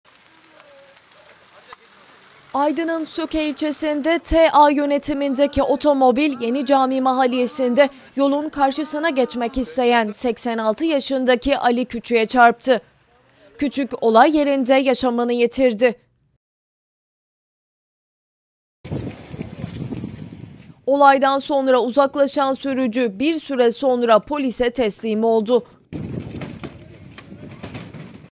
Aydın'da acı olay... Ölüm onu yürürken yakaladı
Aydın'ın Söke ilçesinde otomobilin çarptığı yaya hayatını kaybetti.